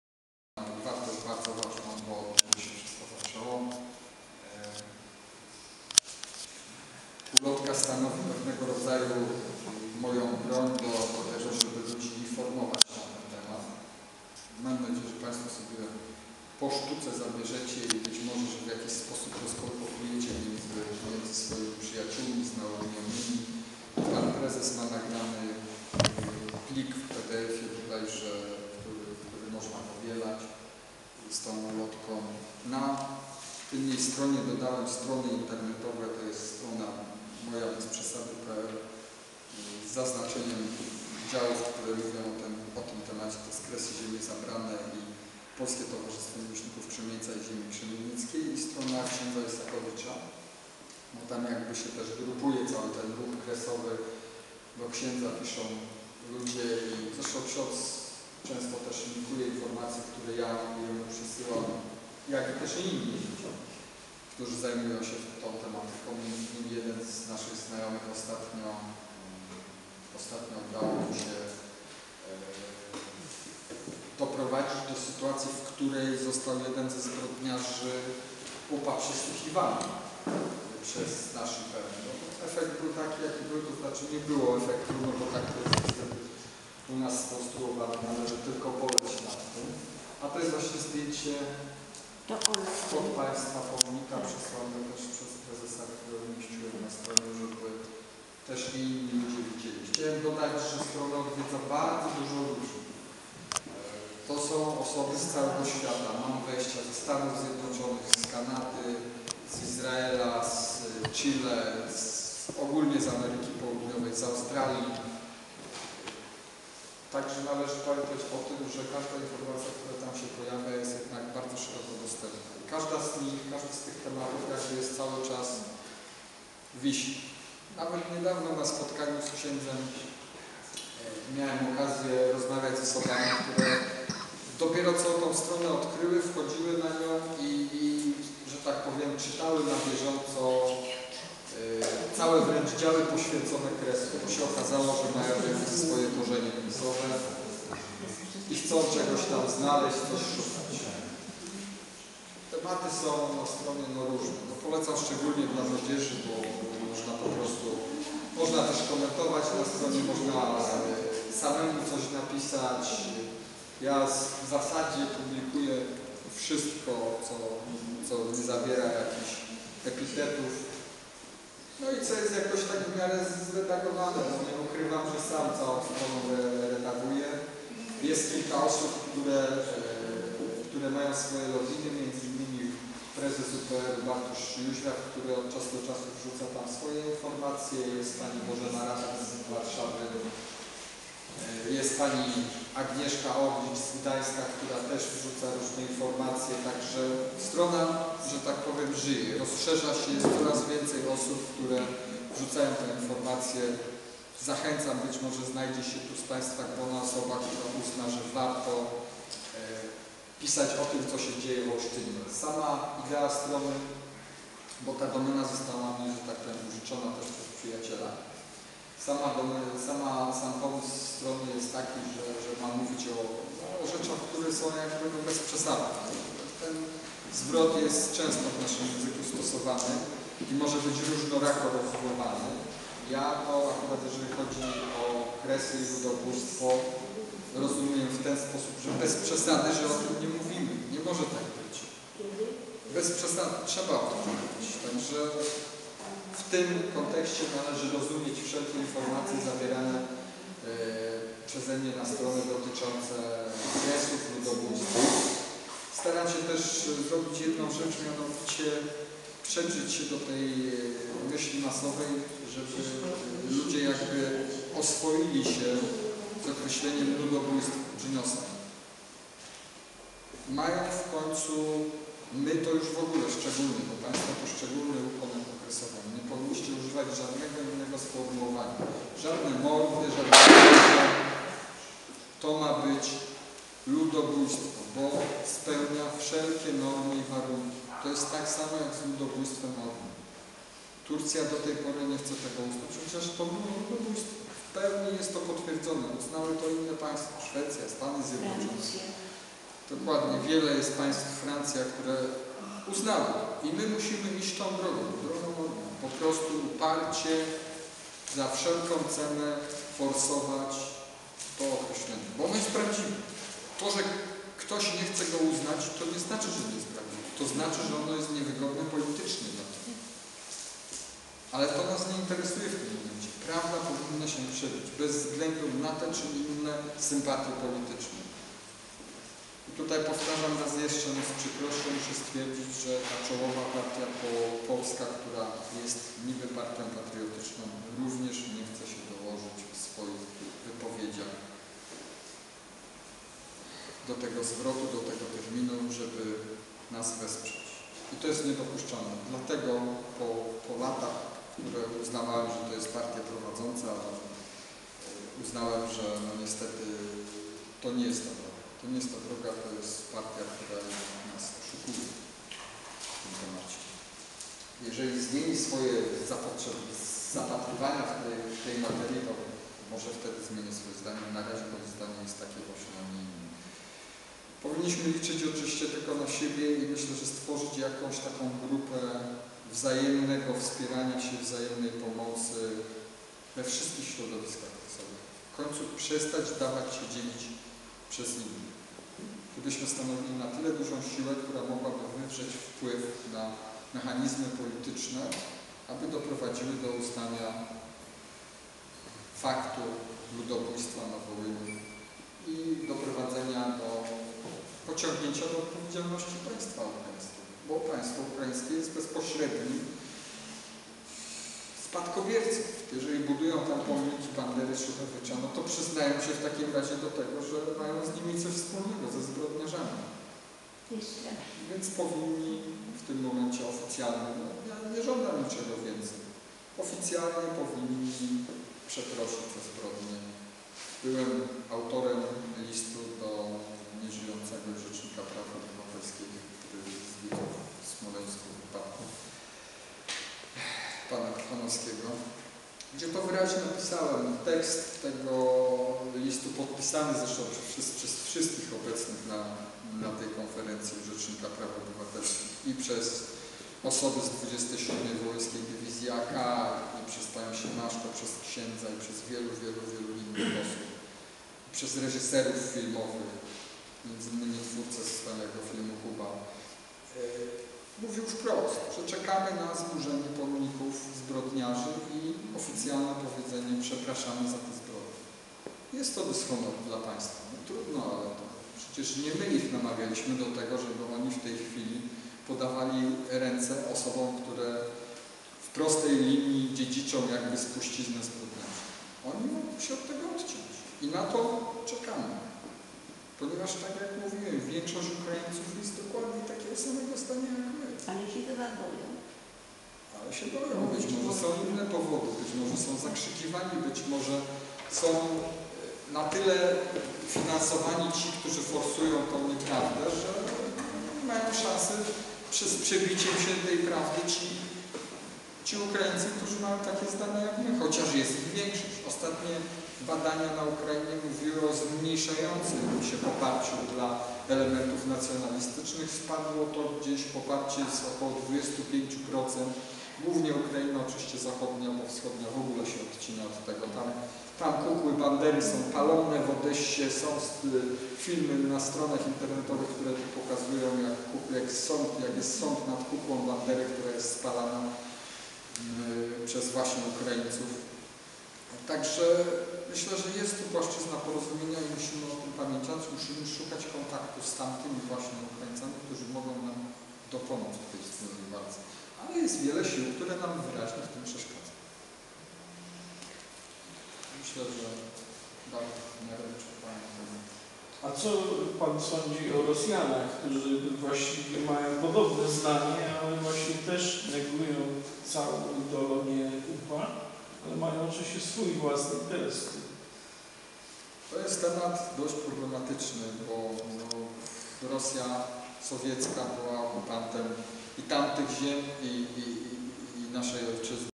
Spotkanie w Olsztynie 2012 Ludobójstwo OUN-UPA - Piotr Szelągowski - cz.1
Spotkanie z działaczem kresowym z Poznania propagatorem Prawdy o Ludobójstwie na narodzie polskim dokonanym przez OUN-UPA.Spotkanie odbyło się w dniu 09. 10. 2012.